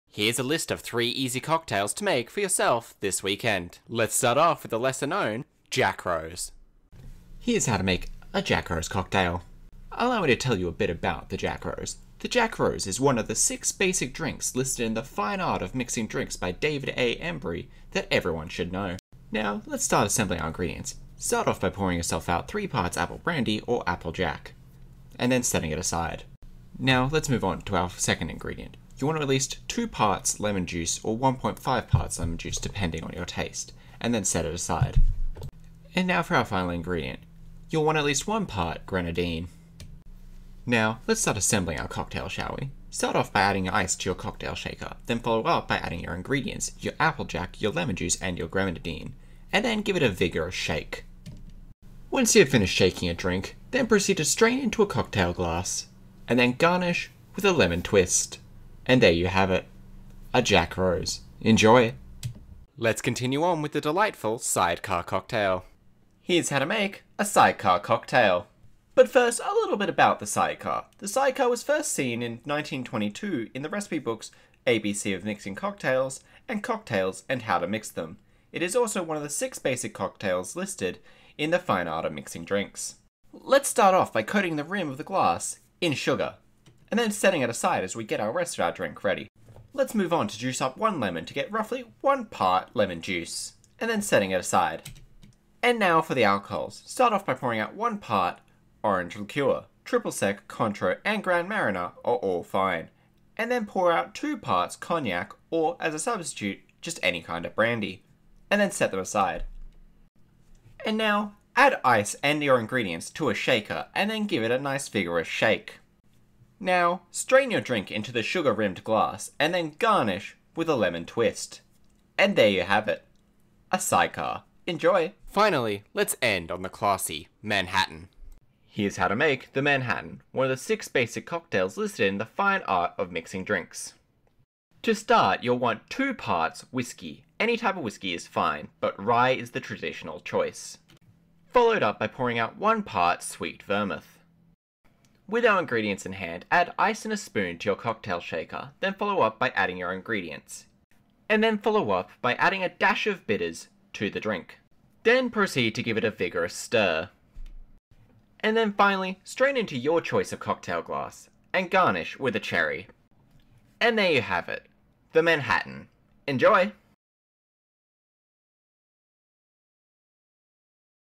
3 of the six basic drinks (Jackrose, sidecar, and manhattan) (Short Compilation)
Here you'll find how to make 3 key cocktails that'll impress everyone at your parties, the sidecar, Jackrose and the Manhattan.

This is just a compilation video of three of the shorts that I have made in the past. Nothing super long, though I am planning on doing these at least twice a month to help showcase my work.

This is mostly to celebrate the 6 months I have been doing this for and hopefully see how these types of videos work compared to shorts and streaming.